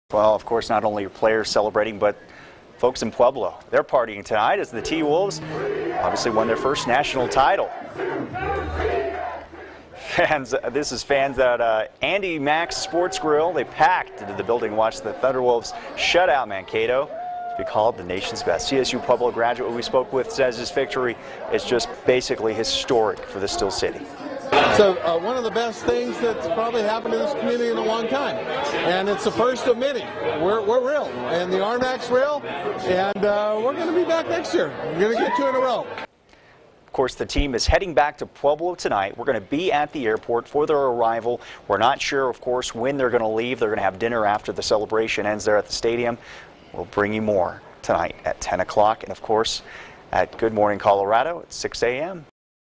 Fans celebrate CSU-Pueblo's first football national title
Pueblo is rocking Saturday night as the Colorado State University-Pueblo ThunderWolves won its first NCAA Division II Football National Championship.